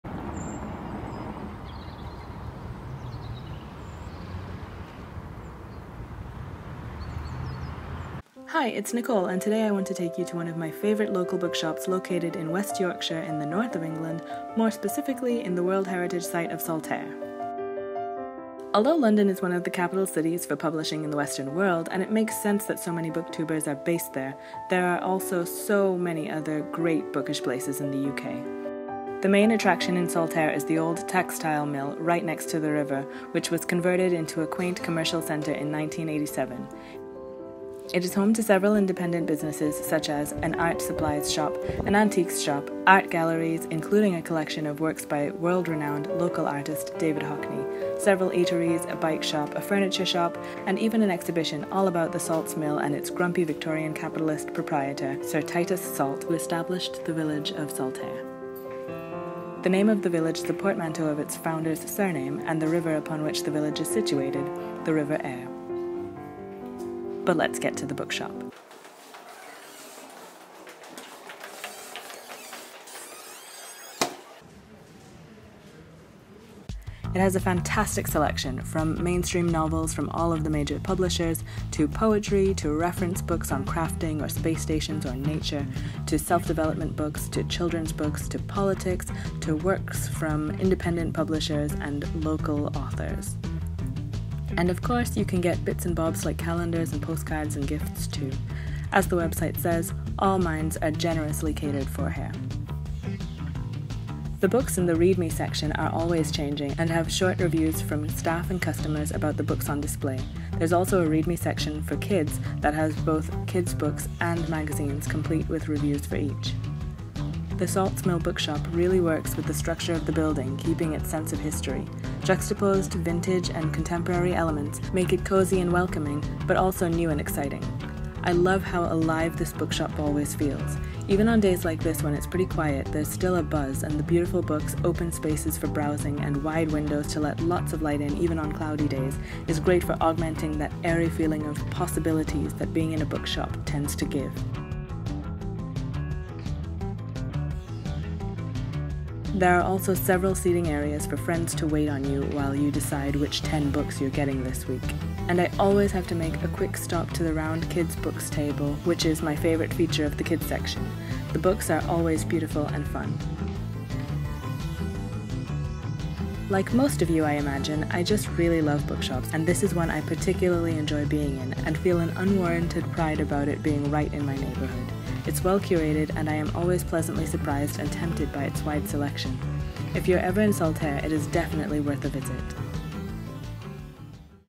Salts Mill Bookshop
I've been sitting on this footage for a month (no exaggeration) because I was so nervous about not doing it justice. I decided to just bite the bullet and do my best. Hopefully you can see why I love this bookshop so much.

On another note, please let me know if there are any major problems with the sound (too loud or too quiet)! I'm really not sure how to judge it as it's loud in my headphones but quiet from my laptop speakers.

Monday to Friday 10.00am to 5.30pm
Saturday and Sunday 10.00am to 6.00pm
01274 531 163